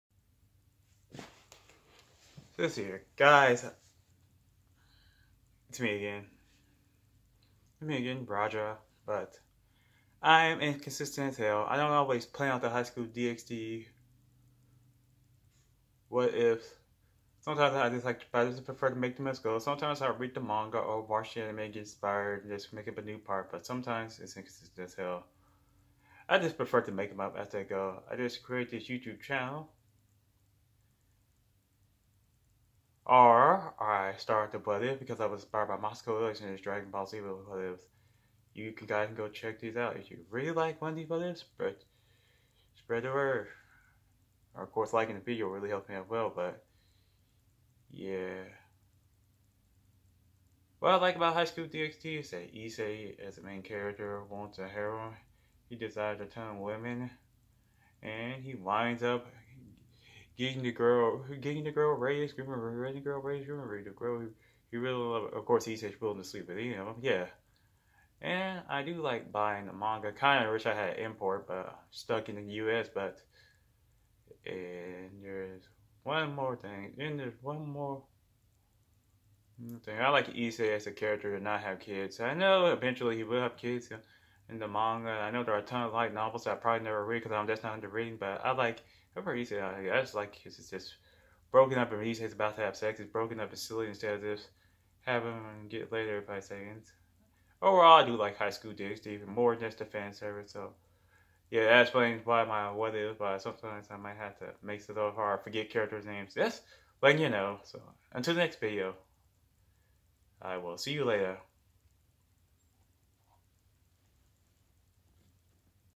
How Do my High School Dxd What Ifs For this Channel
Just Thinking I tell U how
How my what IF work here.
N Why I High School Dxd the series
plaes enjoy the video. later